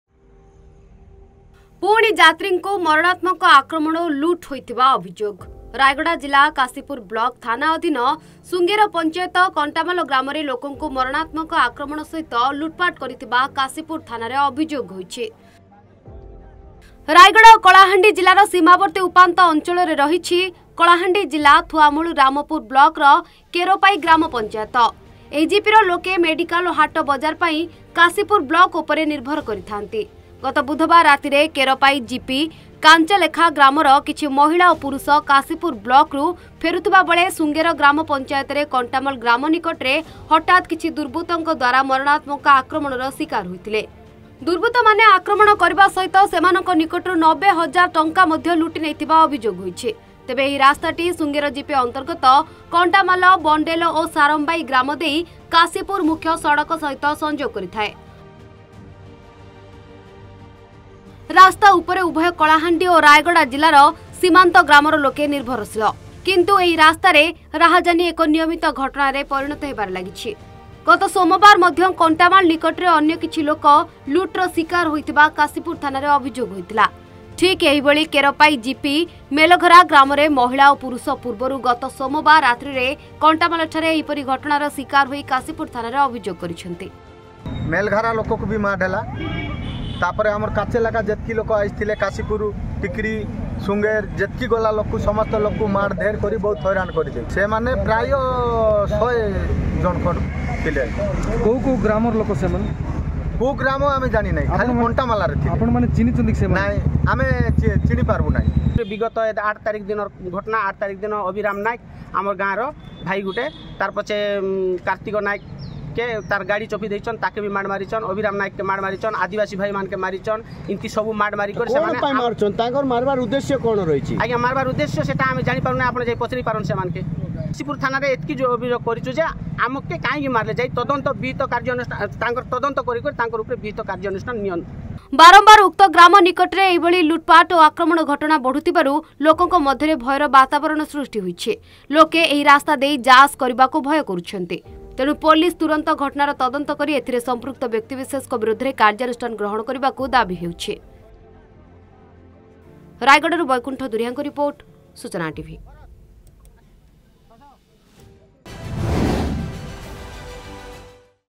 ରାସ୍ତାରେ ବାରମ୍ବାର ହେଉଛି ଆକ୍ରମଣ, କାର୍ଯ୍ୟାନୁଷ୍ଠାନ ପାଇଁ ଦାବି
ବାରମ୍ବାର କଣ୍ଟାମାଳ ନିକଟରେ ଏହି ଭଳି ଲୁଟପାଟ ଓ ଆକ୍ରମଣ ଘଟଣା ଘଟୁଥିବାରୁ ଲୋକଙ୍କ ମଧ୍ୟରେ ଭୟର ବାତାବରଣ ଖେଳିଯାଇଛି। ଲୋକେ ଏହି ରାସ୍ତା ଦେଇ ଯାଆ ଆସ କରିବାକୁ ଭୟ କଲେଣି। ତେଣୁ ପୋଲିସ ତୁରନ୍ତ ଘଟଣାର ତଦନ୍ତ କରି ଏଥିରେ ସମ୍ପୃକ୍ତ ବ୍ୟକ୍ତିବିଶେଷଙ୍କ ବିରୋଧରେ କାର୍ଯ୍ୟାନୁଷ୍ଠାନ ଗ୍ରହଣ କରିବାକୁ  ଦାବି ହେଉଛି।

Dear Viewers,

ccits